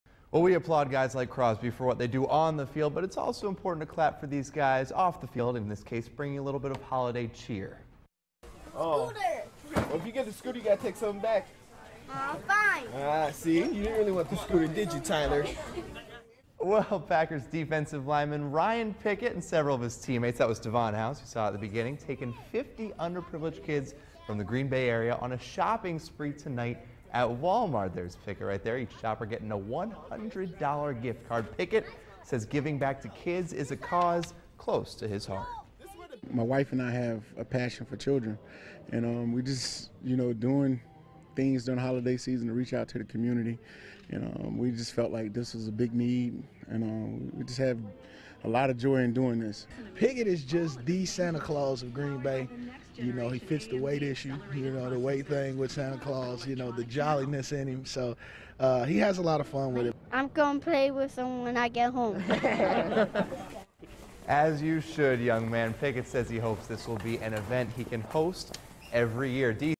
Pickett takes kids on shopping spree
Packers teammates join in to help.